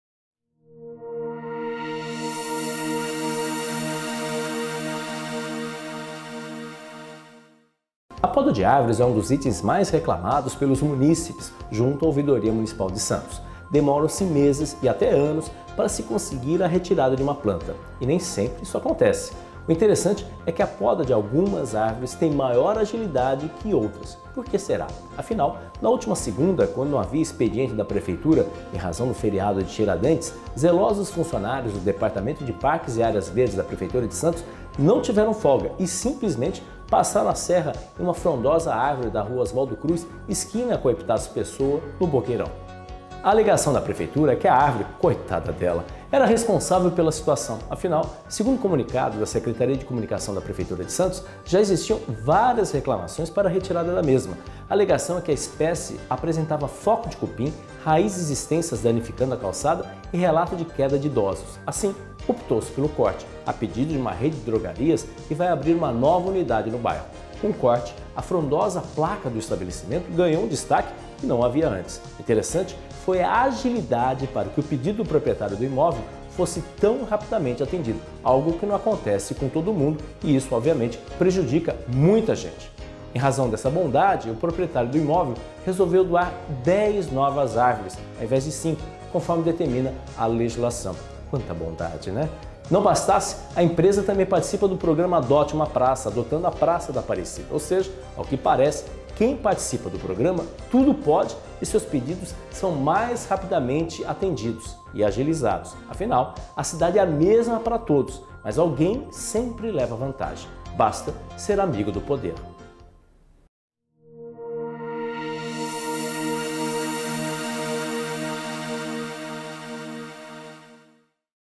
BoqnewsTV - Podas de Árvores em Santos
As podas de árvores é um dos itens que mais recebem reclamações junto à ouvidoria em Santos. Entretanto, algumas podas ou a retirada de árvores, tem mais prioridades que outras na Cidade. Confira a análise do editor Fernando De Maria aqui na BoqnewsTV.